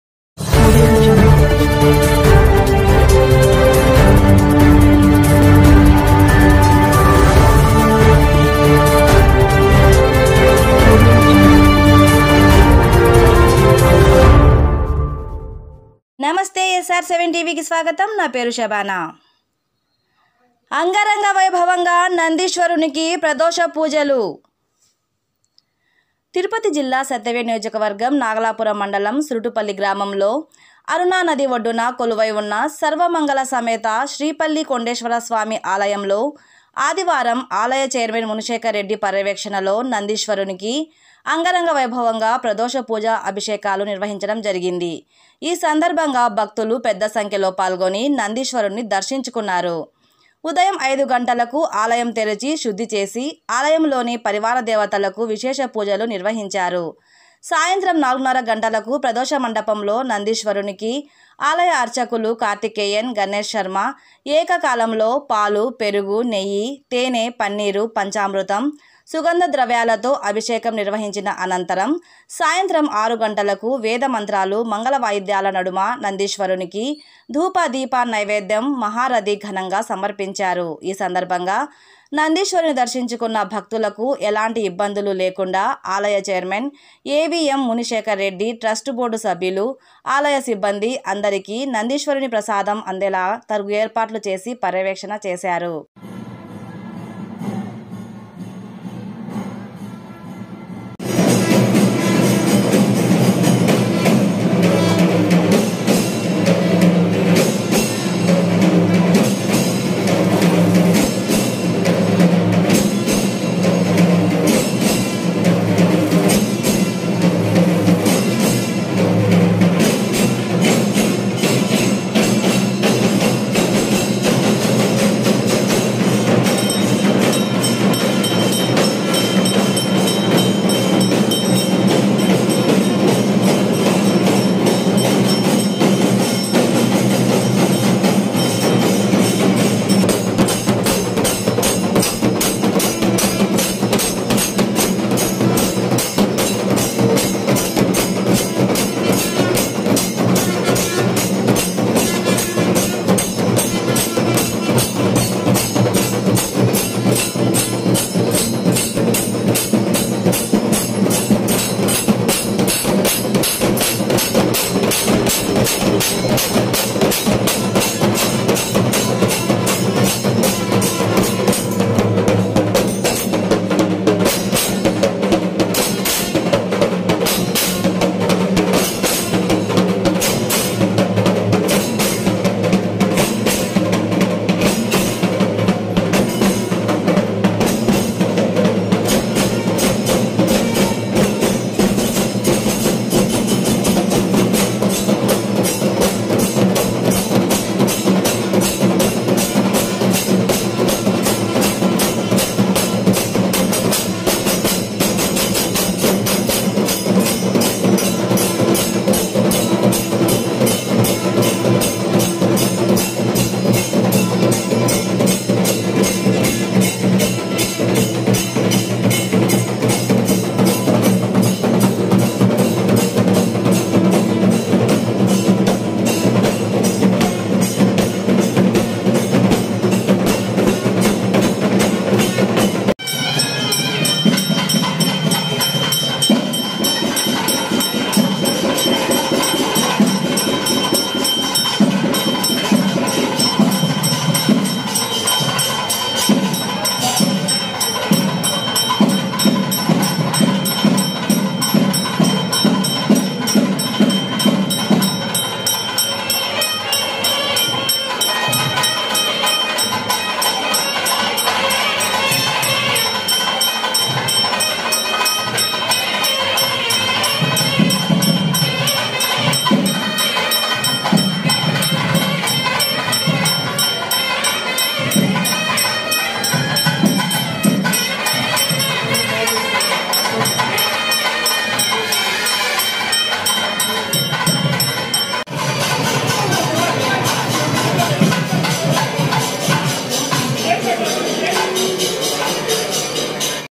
అంగరంగ వైభవంగా నందీశ్వరునికి ప్రదోష పూజలు తిరుపతి జిల్లా సత్యవేనాగలాపురం మండలం సురుటుపల్లి గ్రామంలో.
అంగరంగ వైభవంగా నందీశ్వరునికి ప్రదోష పూజ లు. తిరుపతి జిల్లా సత్యవేనాగలాపురం మండలం సురుటుపల్లి గ్రామంలో అరుణా నది ఒడ్డున కొలువైవున్న సర్వమంగళ సమేత శ్రీ పల్లి కొండేశ్వర స్వామి ఆలయంలో ఆదివారం ఆలయ చైర్మన్ ముని శేఖర్ రెడ్డి పర్యవేక్షణలో నందీశ్వరునికి అంగరంగ వైభవంగా ప్రదోష పూజ అభిషేకాలు నిర్వహించడం జరిగింది ఈ సందర్భంగా భక్తులు పెద్ద సంఖ్యలో పాల్గొని నందీశ్వరుని దర్శించుకున్నారు ఉదయం 5 గంటలకు ఆలయం తెరచి శుద్ధి చేసి ఆలయంలోని పరివార దేవతలకు విశేష పూజలు నిర్వహించారు సాయంత్రం 4:30 గంటలకు ప్రదోష మండపంలో నందీశ్వరునికి ఆలయ అర్చకులు కార్తికేయన్ గణేష్ శర్మ ఏకకాలంలో పాలు పెరుగు నెయ్యి తేనె పన్నీరు పంచామృతం సుగంధ ద్రవ్యాలతో అభిషేకం నిర్వహించిన అనంతరం సాయంత్రం 6 గంటలకు వేద మంత్రాలు మంగళ వాయిద్యాల నడుమ నందీశ్వరునికి దూప దీప నైవేద్యం మహారథి ఘనంగా సమర్పించారు ఈ సందర్భంగా నందీశ్వరుని దర్శించుకున్న భక్తులకు ఎలాంటి ఇబ్బందులు లేకుండా ఆలయ చైర్మన్ ఎ. వి. యం.ముని శేఖర్ రెడ్డి ట్రస్ట్ బోర్డు సభ్యులు ఆలయ సిబ్బంది లు అందరికీ నందీశ్వరుని దర్శనం ప్రసాదం అందేలా తగు ఏర్పాట్లు చేసి పర్యవేక్షణ చేశారు